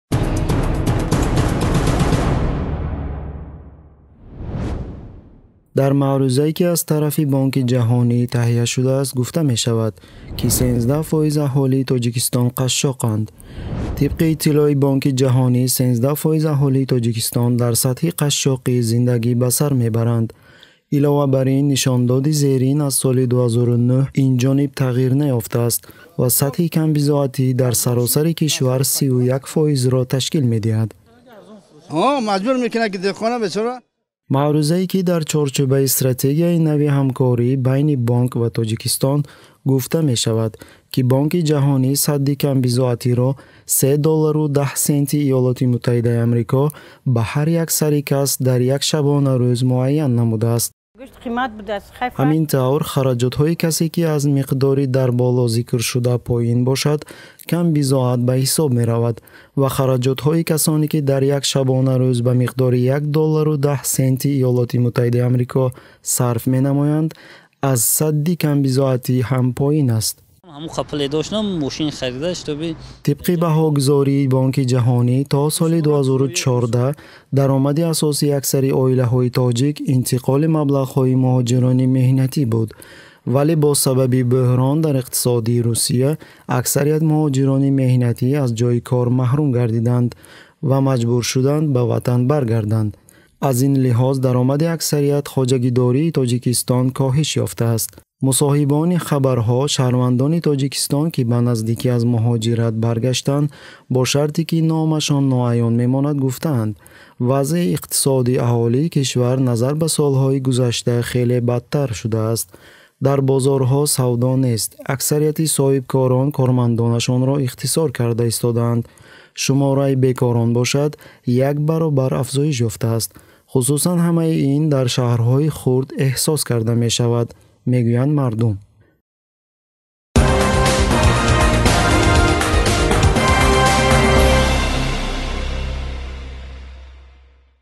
13% аҳолии Тоҷикистон дар сатҳи қашшоқӣ зиндагӣ ба сар мебаранд
Дар маърӯзае, ки аз тарафи Бонки ҷаҳонӣ таҳия шудааст гуфта мешавад, ки 13% аҳолии Тоҷикистон қашшоқанд.